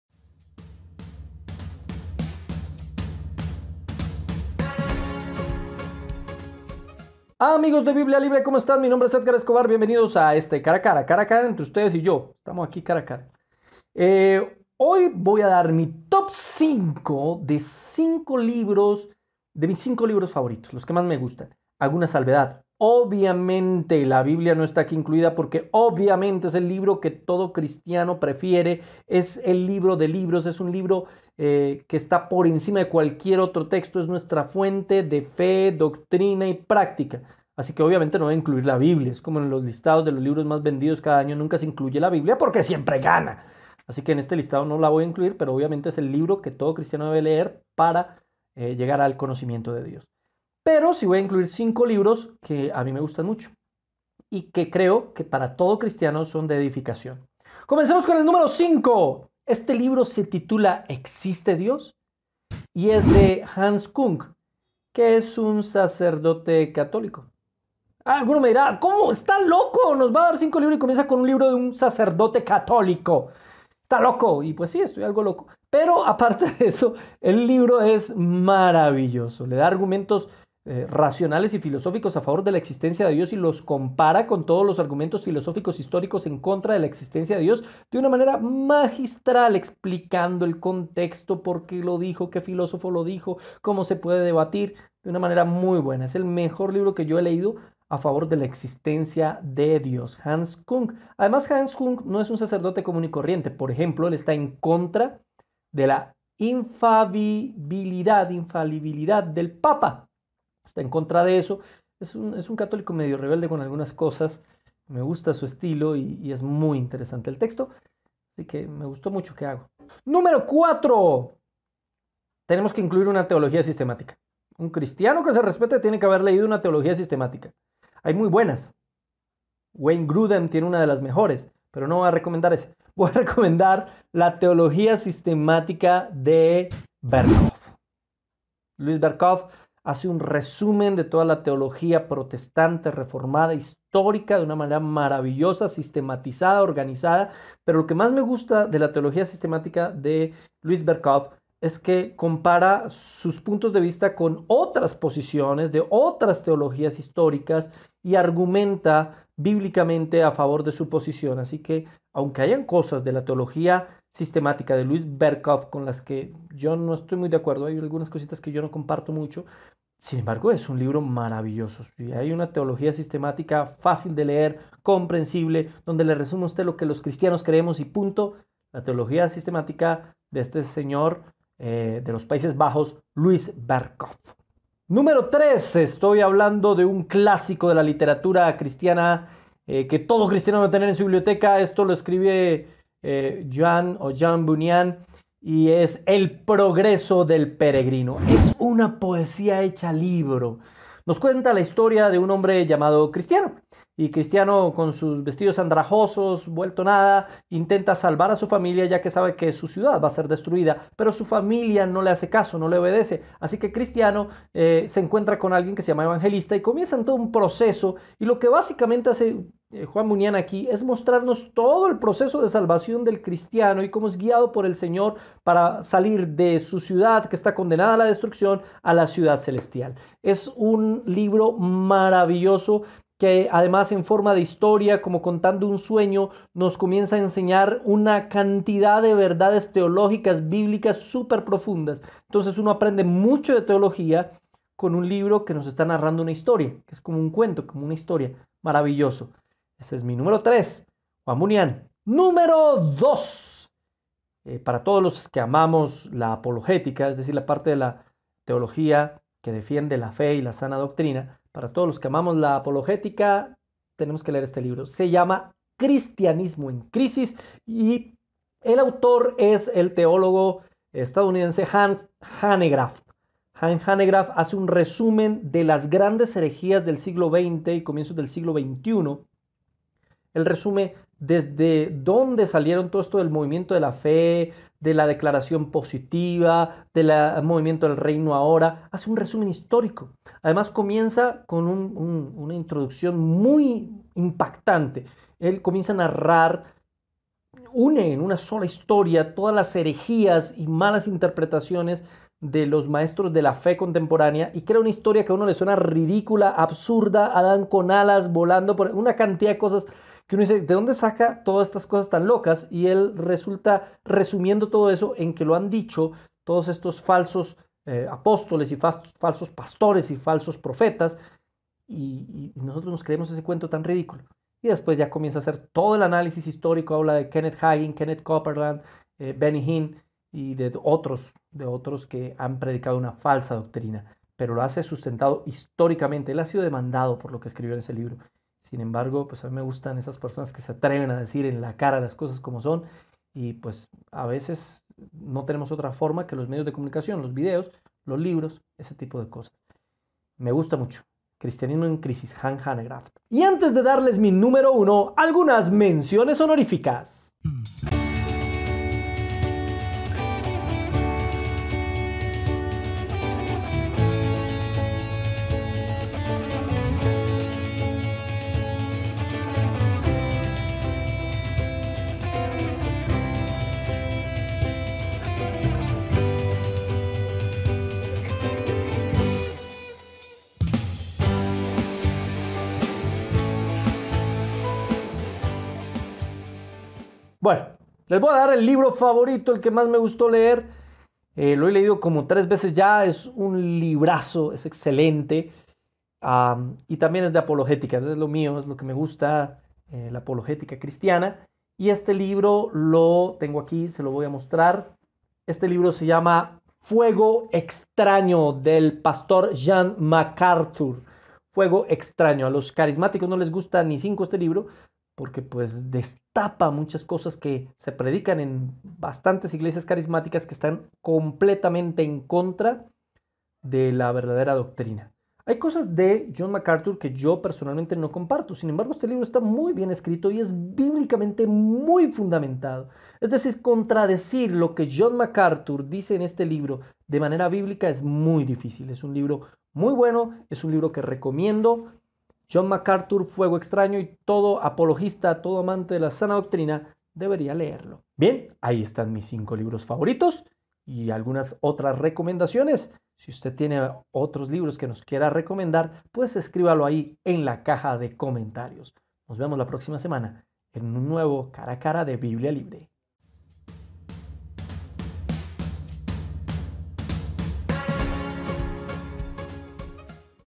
5 LIBROS CRISTIANOS QUE RECOMIENDO... Y UNOS MÁS
La Biblia es única e incomparable, pero hay muy buenos textos que nos acercan a ella, aquí mi top 5 de textos cristianos.